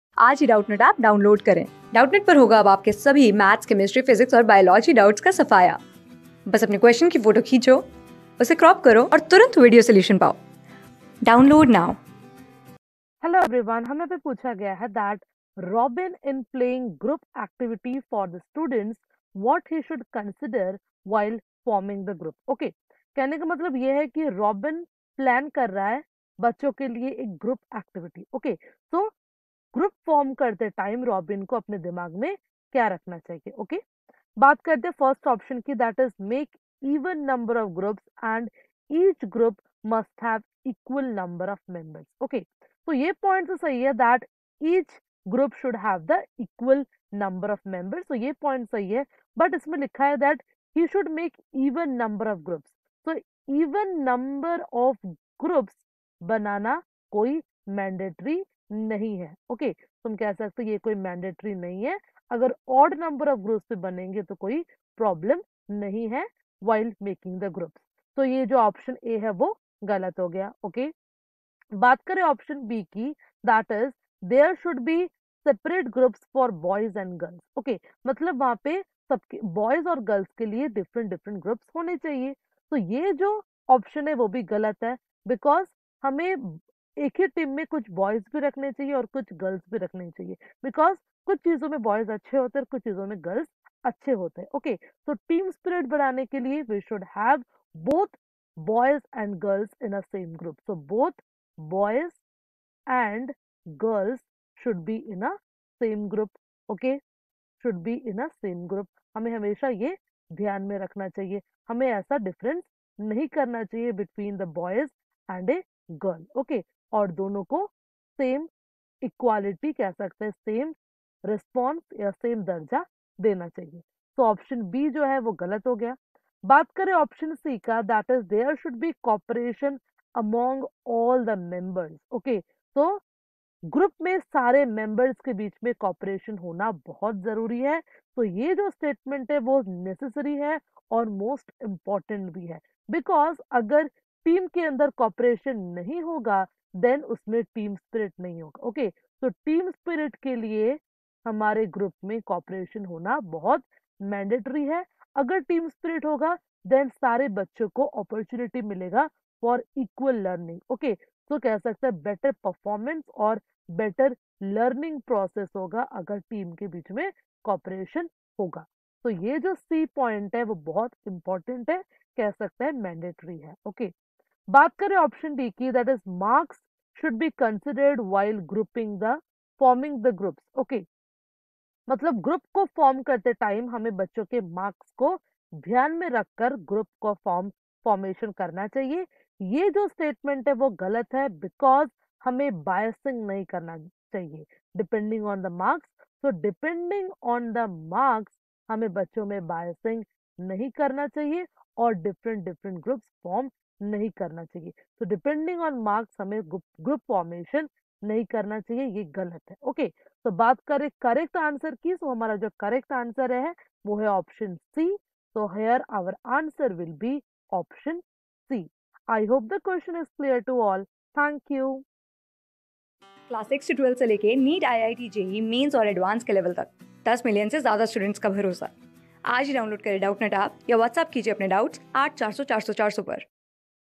Robin in planning group activity for students what he should consider while forming the group ? ...
Class: 14
Subject: ENVIRONMENTAL STUDIES
Chapter: ACTIVITY AND EXPERIMENTATION
Board:CTET

👉 Have Any Query? Ask Us.